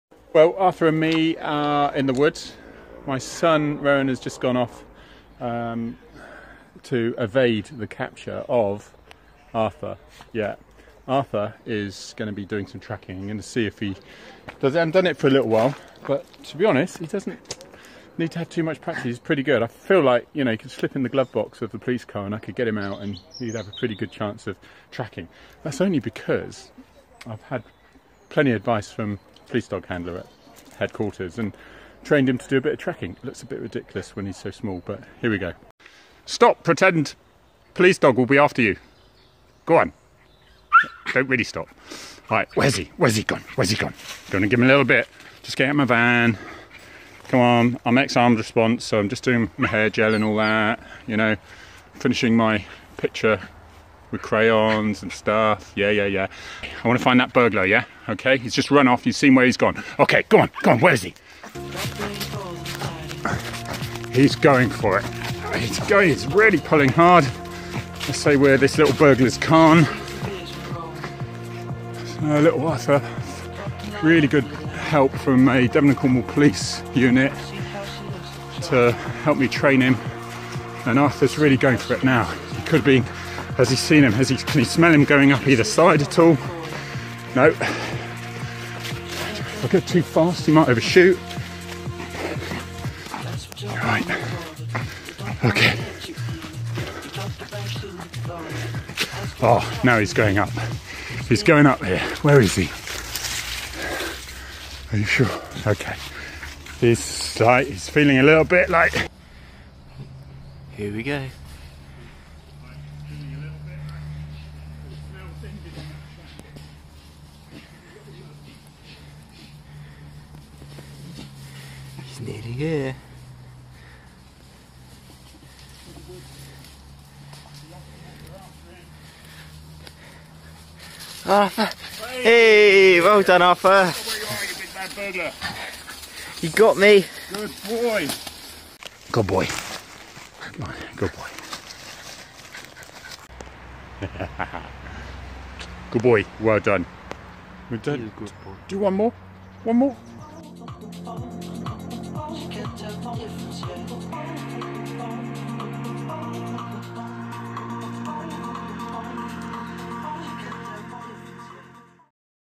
Arthur the Border Terrier / Jack Russell Cross tracks a criminal!
This is Arthur who has been trained to track people just like a police dog. He doesn't have size on his side for dealing with them when he finds them but he has plenty of attitude.  You can follow his master Harry Tangye on Twitter at @DC_ARVSgt